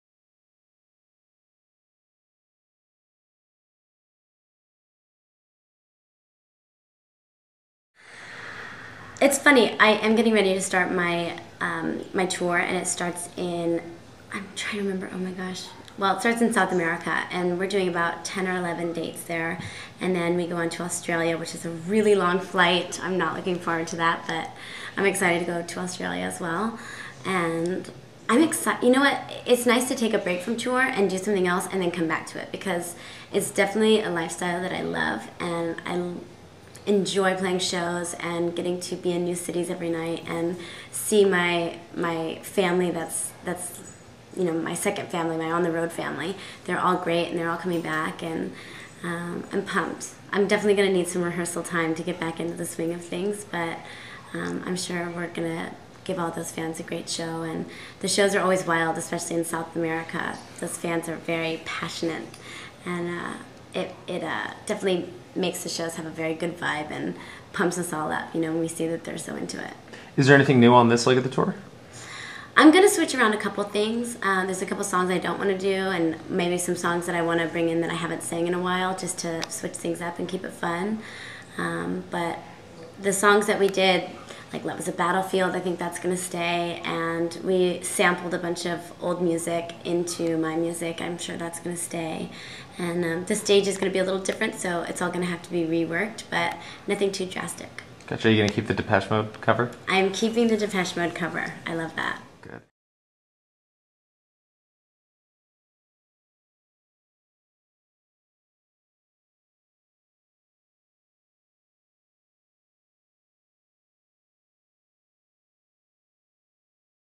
The Hilary Duff Video Podcast - Back on the Road
Hilary talks about going back on the road for her tour through Latin America and Australia!

Go to HilaryDuff.com for more!